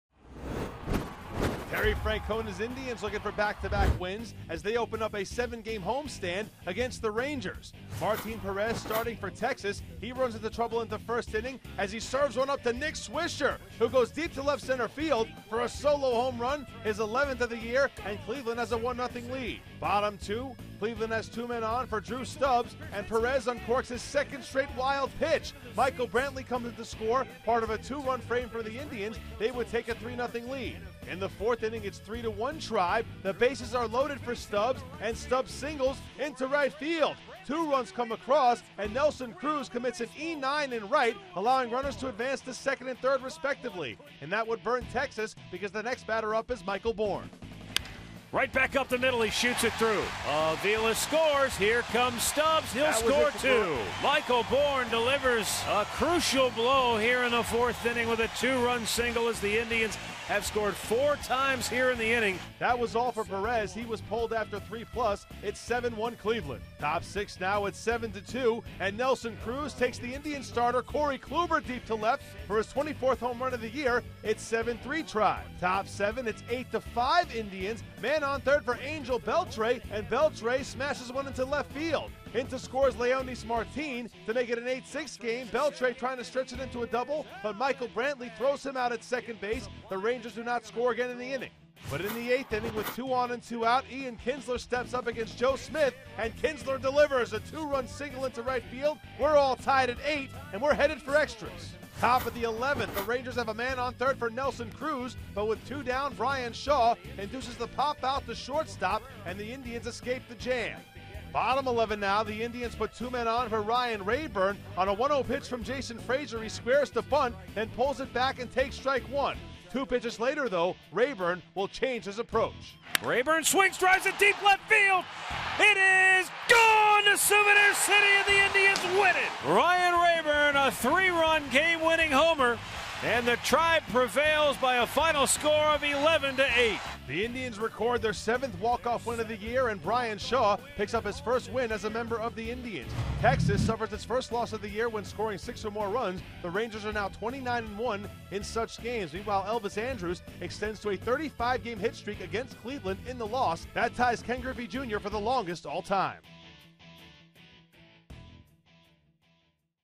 7/26/13: Raburn goes deep to walk off in extras
Daily Recap: Ryan Raburn hit a three-run walk-off home run in the 11th to send the Indians to an 11-8 win after blowing a 7-1 lead